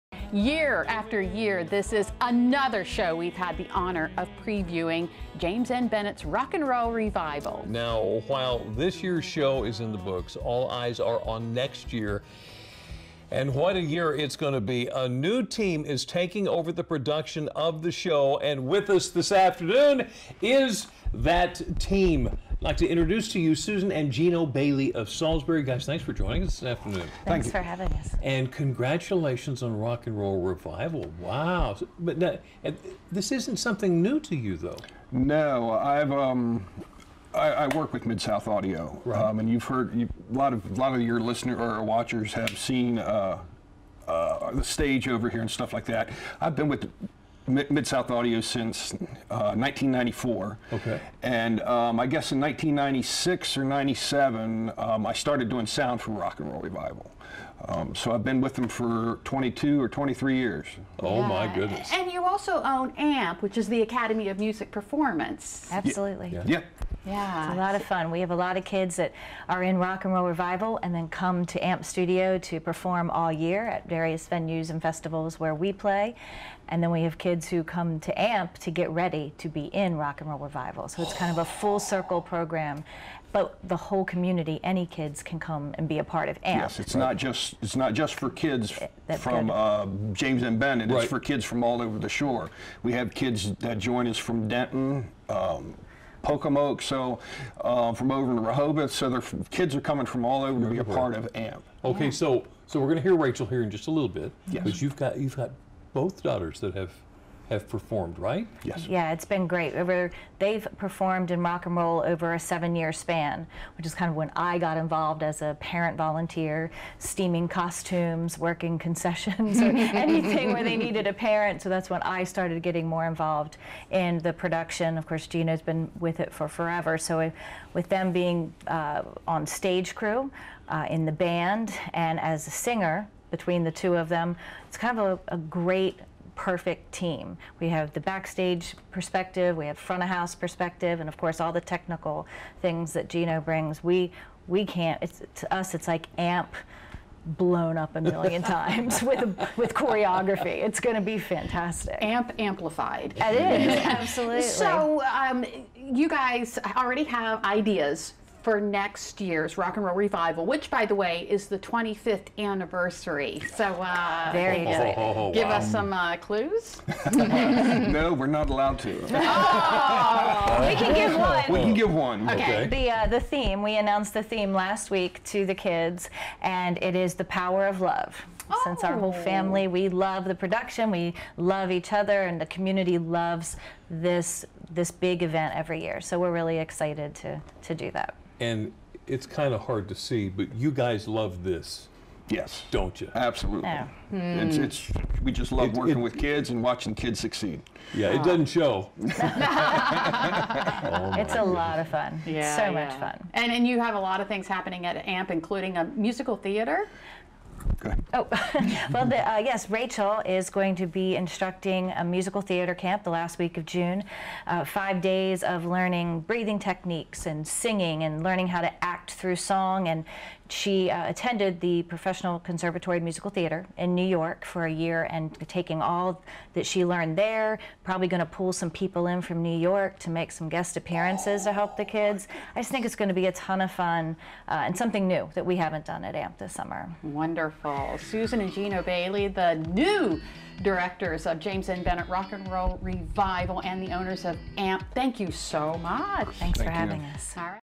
We Get Some News About Next Year's Rock n' Roll Revival Show
James M. Bennett High School's Rock n' Roll Revival is in the books for this year, but we have exciting news to share about next year's show.